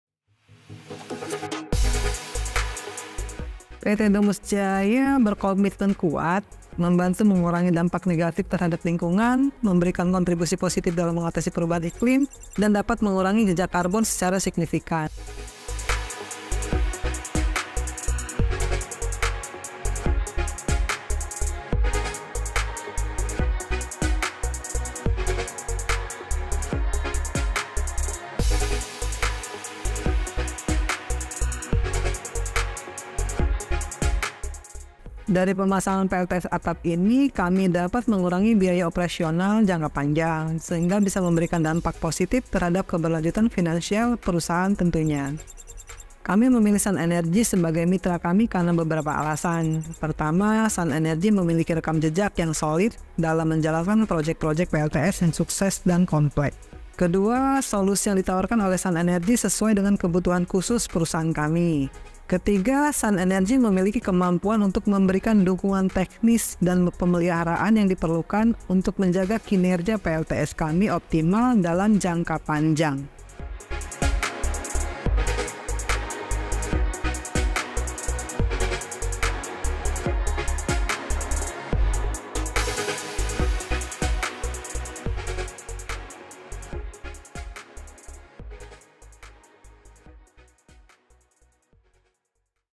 PT Domus Jaya Gunakan PLTS Atap untuk Produksi Minyak
PT Domus Jaya di Lampung Selatan kini sudah memanfaatkan energi terbarukan yang lebih ramah lingkungan. Sebagai perusahaan refinery besar di wilayah Lampung, PT Domus Jaya mengambil langkah besar menuju masa depan berkelanjutan di Indonesia.

Bekerja sama dengan SUN Energy, PT Domus Jaya memasang sistem energi surya yang akan melistriki bangunan produksinya. Langkah ini tidak hanya menguntungkan perusahaan saja, namun juga sebagai bukti positif dalam mendukung implementasi prinsip keberlanjutan di sektor bisnis dengan mengurangi dampak lingkungan yang dihasilkan.

Mari dukung perusahaan-perusahaan seperti PT Domus Jaya yang telah berkomitmen untuk beralih ke energi terbarukan. Bersama, wujudkan visi Indonesia yang lebih hijau.
PT Domus Jaya in Southern Lampung is now utilizing renewable energy that is greener for the environment. As a prominent refinery company in the Lampung region, PT Domus Jaya is taking a significant step towards a sustainable future in Indonesia.

In collaboration with SUN Energy, PT Domus Jaya installed a solar energy system that will power its production plant. This step is not only beneficial for the company but also a positive proof in supporting the implementation of sustainability principles in the business sector by reducing its environmental impact.

Let's support companies like PT Domus Jaya that have committed to switching to renewable energy. Together, we are able to fulfill the vision of a greener Indonesia.

Bentuk masa depan yang lebih berkelanjutan dan ramah lingkungan dengan energi surya. Temukan solusi sistem energi surya terintegrasi untuk bisnis industri dan komersial Anda bersama SUN Energy